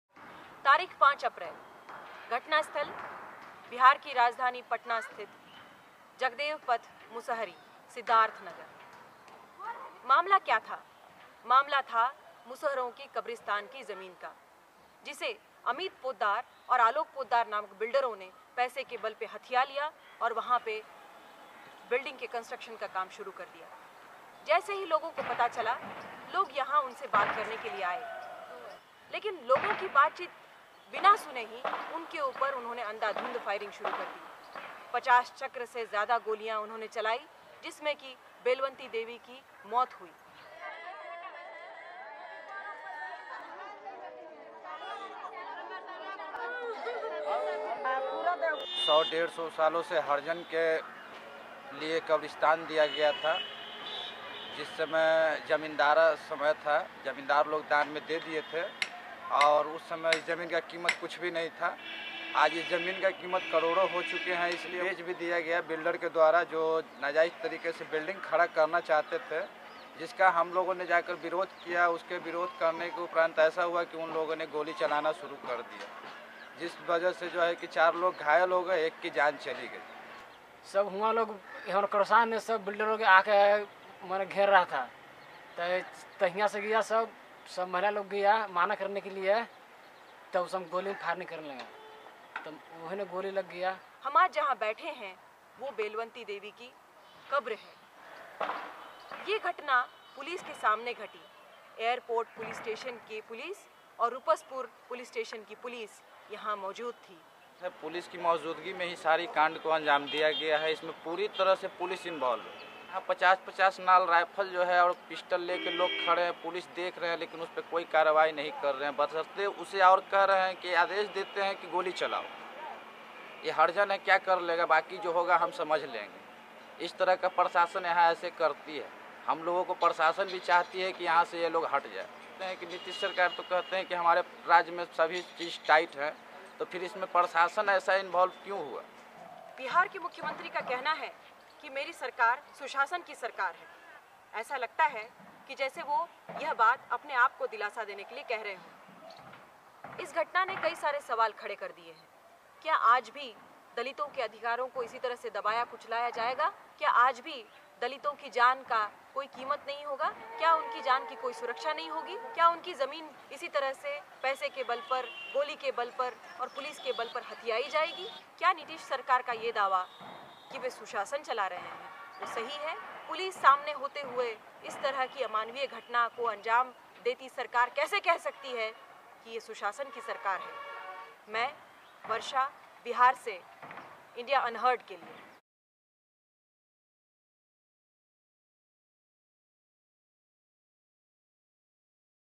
Land and Life Lost in Bihar.
A young woman dies and frustrations rise over a land grab attempt in a Dalit community in Bihar.

On April 5th, Belwanti Devi was shot to death during a protest in Patna, Bihar. The protest was spurred by a land grab in a Mussahar Dalit community where she lived with her family. When Jai Niwas Constructions (aka Amit Constructions) began the process of erecting a building on the community graveyard, Devi joined her neighbors and marched on the site; where they were greeted with rounds of bullets from developers. Days of protests and violence ensued.

The death of Belwanti Devi and the land grabs that have enraged the Mussahar community have received minimal press attention in the region. Such a blatant misuse of power resulting in the death of a young woman is yet another example of India's Unheard tragedies. Reporting from Devi's gravesite, our community correspondent in Bihar, Varsha Jawalgekar, brings us this story from the mouths of the community members themselves.

At present, the Mussahar community in Patna stands to lose what little communal land they have in India's wide spread landing grabbing trend. "For the last 150 years the land has been used as a graveyard," said community member Arun Manjhi. However, when the land was originally donated to the community it was worth very little. Now, as land becomes a sought after commodity in Bihar, it is valued in the millions and has been illegally seized by developers Amit and Alok Paddar.

Community witnesses maintain that during the incident, area police stood by and made no efforts to stop the violence or to protect unarmed protesters. Many believe that corrupted police are working with the land grabbers. One man stated that police justify their inaction because "they are Dalits, [and so] there is little they can do." Although community activism and increased public pressure following the incident resulted in the suspension of a leading officer and the arrest of Amit Paddar, the community is not convinced that area police can be trusted.

The Indian government has been openly criticized for its role in the land grabbing and forced eviction issues and many land reform laws are currently under review. Yet, speaking with conviction just after the incident, Arun Manjhi said, "the government wants us Dalits to vacate this land." This comment is telling, especially in Bihar where Chief Minister Nitish Kumar has been often praised for his attention to caste issues and accountability in the police force. In this way, the incident on April 5th has exposed the disparity between claims of good governance by Bihar officials and the experiences of this Dalit community.

For the moment, as the investigation continues, Amit Constructions has ceased building on the community graveyard and local community activists have promised to oppose any further construction. But, in the absence of a visit from CM Kumar in the wake of this looming tragedy, our community correspondent Varsha asks, "Will the rights of Dalits continue to be suppressed and trampled on?"